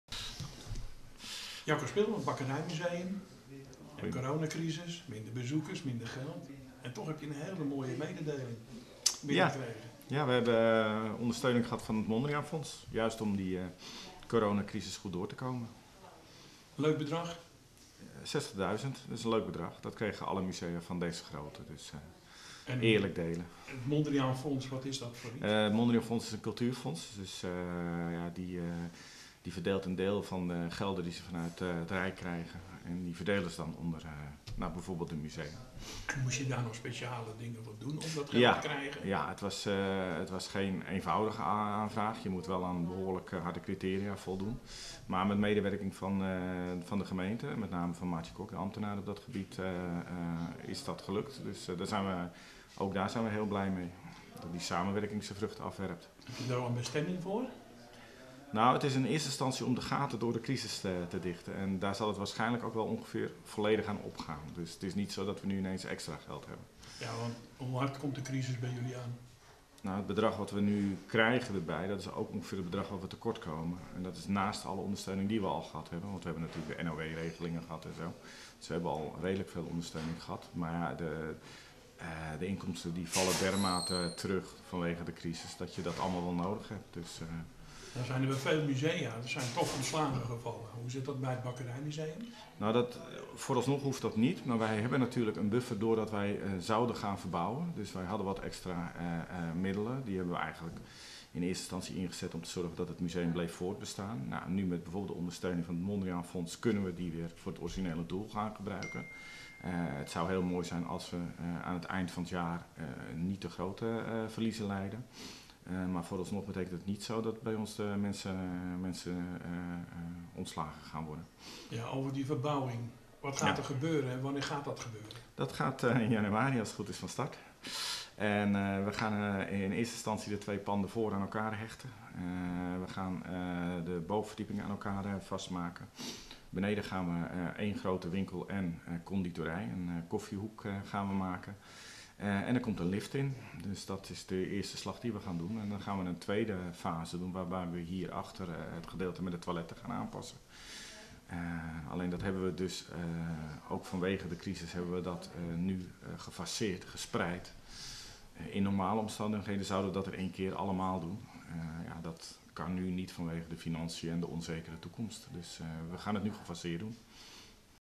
60 000 euro voor het Bakkerijmuseum
MEDEMBLIK - Jacco Spil, directeur van het Bakkerijmuseum in Medemblik, is blij met de steun van het Mondriaan Fonds. Het Mondriaan Fonds heeft besloten om het museum een bijdrage geven van 60.000 euro in deze coronacrisis..